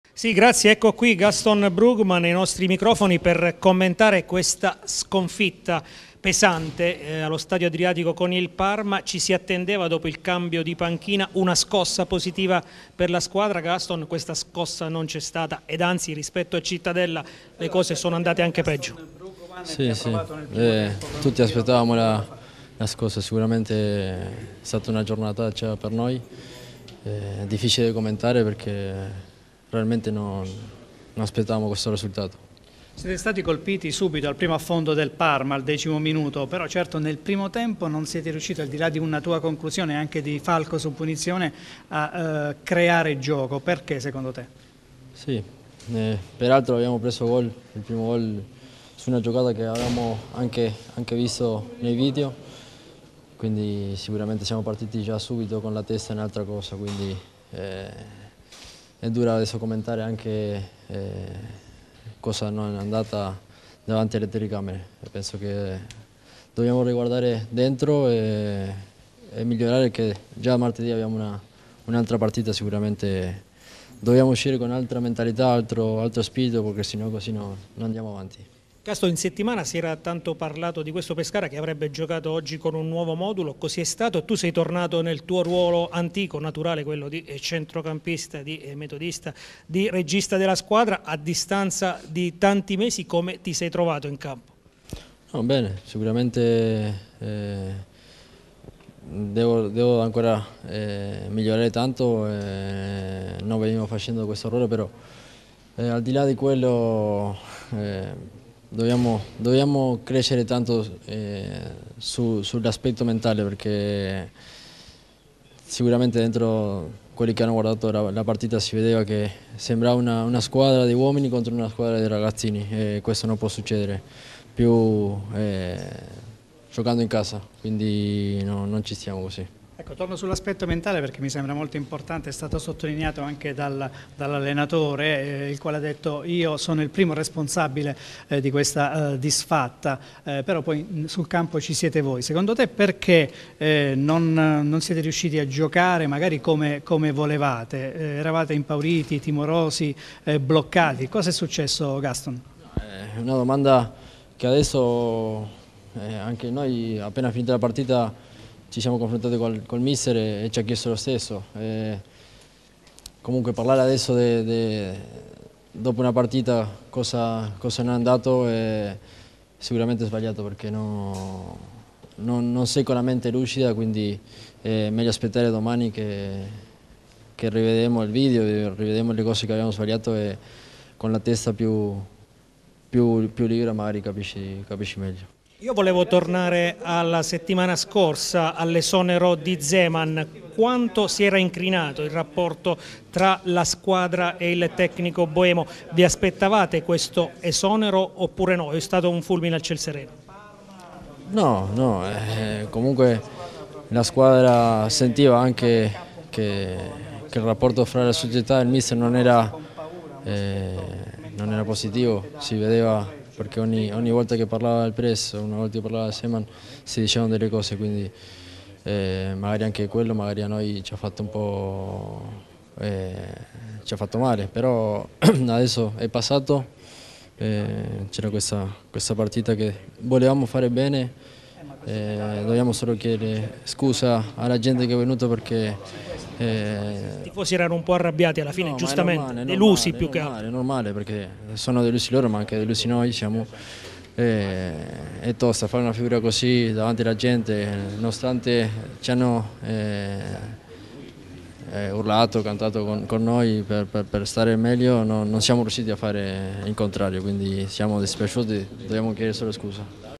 Pescara - Parma 1-4: Gaston Brugman
Intervista post gara al capitano del Pescara